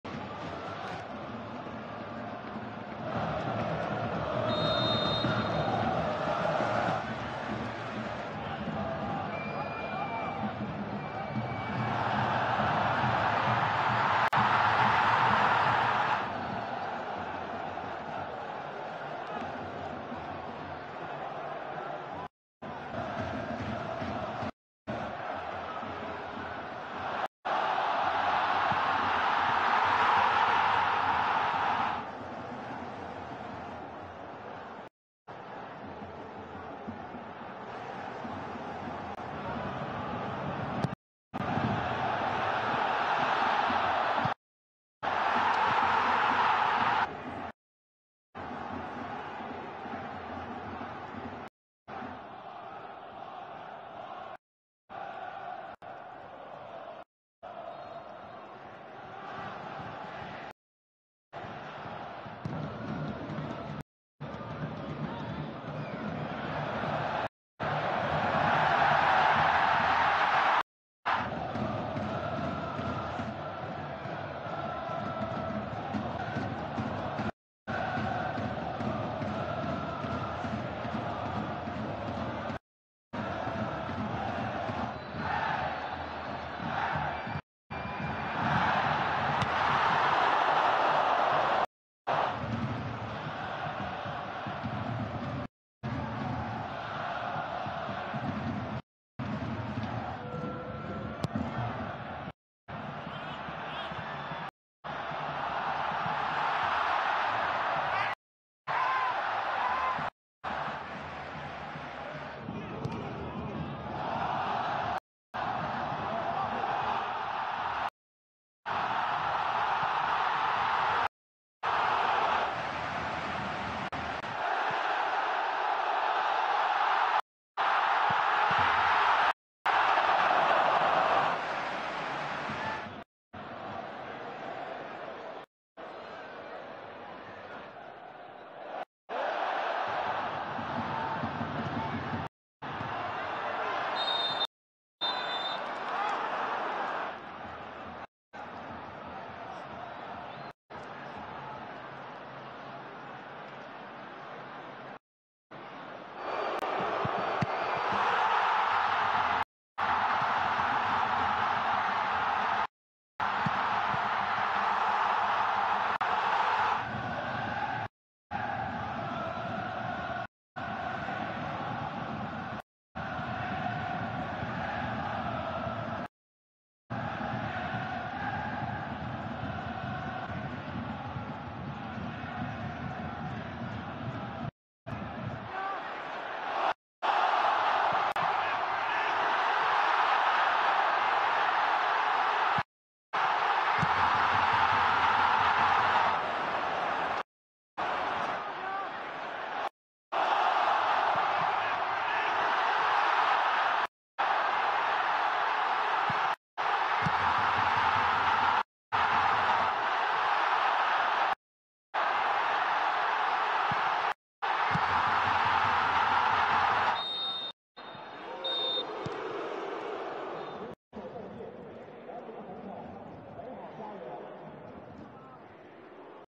[CSL]김신욱 멀티골! 상하이 선화, 우한 줘얼전 전반 2-1 종료
최강희 감독이 이끄는 상하이 선화는  2일 오후 상하이 훙커우 축구장에서 열린 중국 슈퍼리그 21라운드 홈 경기에서 우한 줘얼에 전반전을 2-1로 앞서며 마쳤다. 상하이 선화는 김신욱이 32분과 44분에 골을 터트렸다.